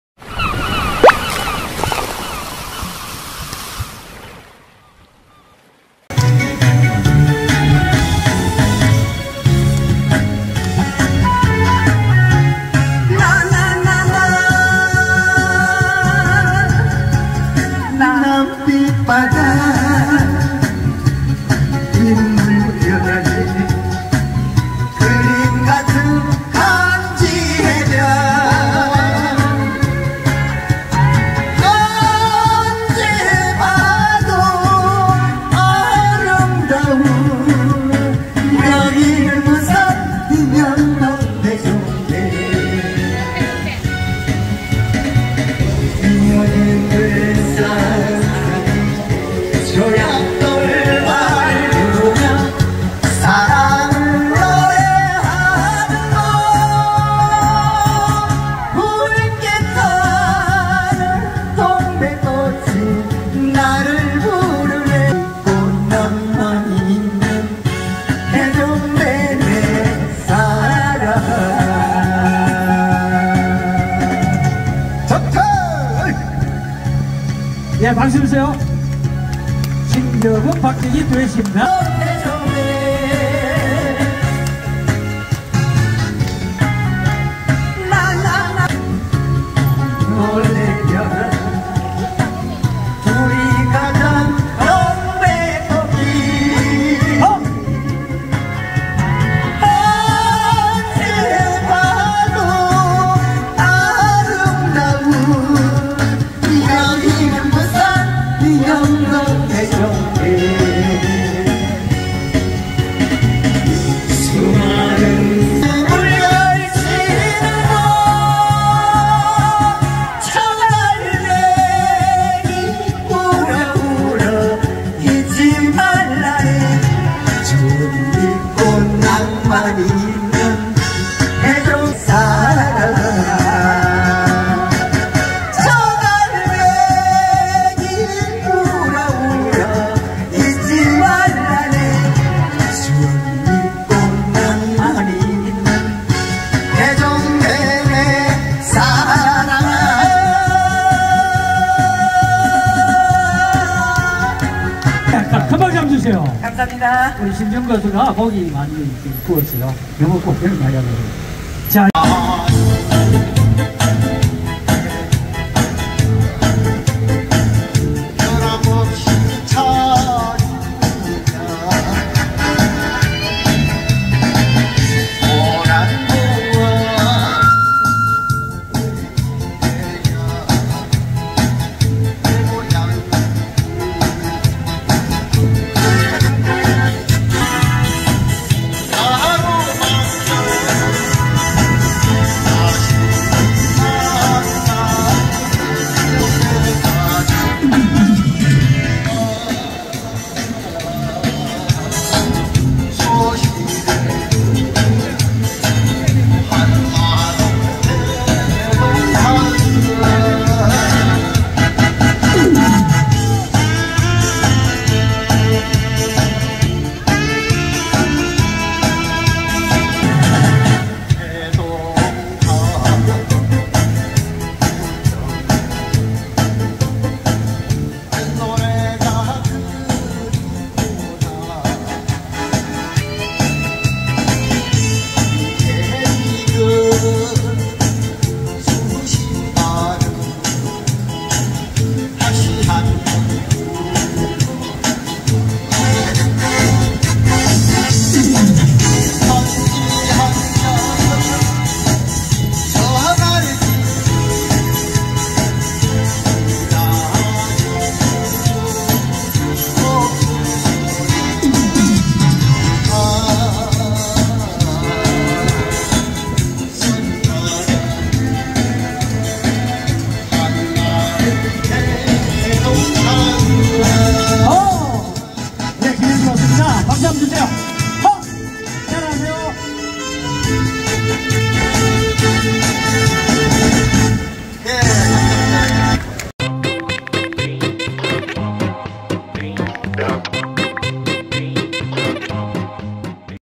7080 버스킹 서면문화거리 그린연예예술단
문화의거리 서면영광도서앞 그린연예예술단원 버스킹 3시간공연
훈훈하고 옛추억으로 출연진 30여명
감사합니다.